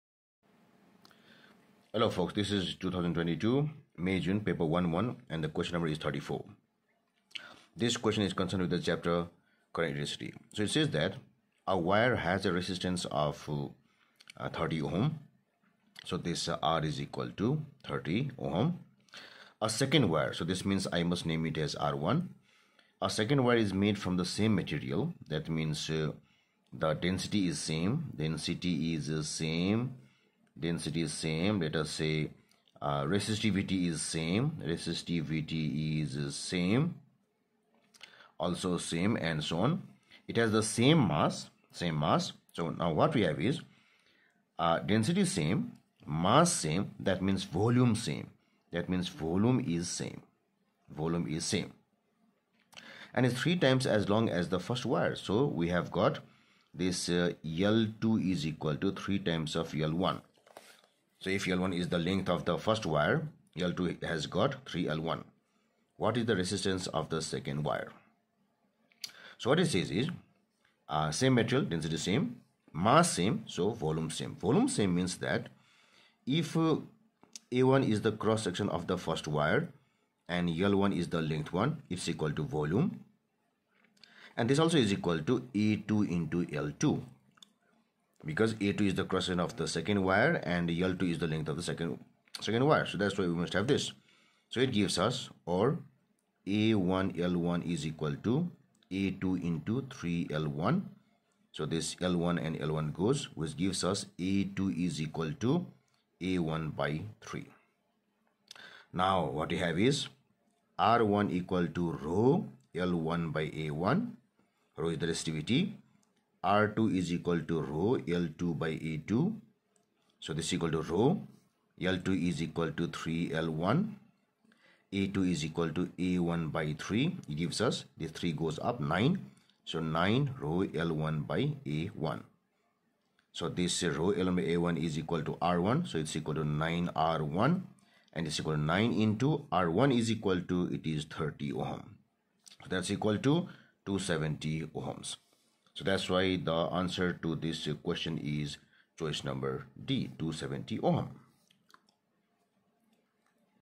2022 CAIE AS & A level May June Physics Paper 11 Q N 34 (9702/11/M/J/22) by Sajit C Shakya
2022 CAIE AS & A level May June Physics Paper 11 Q. No. 34 (9702/11/M/J/22) by Sajit Chandra Shakya from Nepal

A wire has a resistance of 30. A second wire is made from the same material, has the same mass and is three times as long as the first wire.

What is the resistance of the second wire?